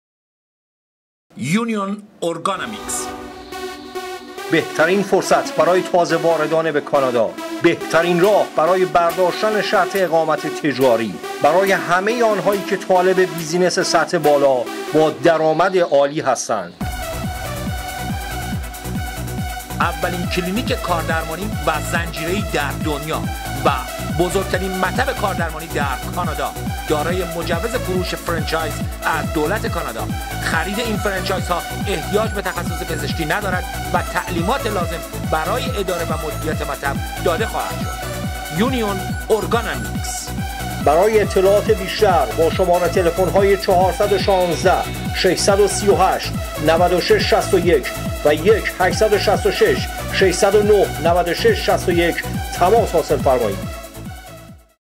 Union Ergonomics 1st ad in Ten TV - 2009
Union Ergonomics (founded by Dr Shahin Pourgol) is the number one provider of ergonomics assessments and supplies to Canadian workers as well as Canada's fastest growing new franchise.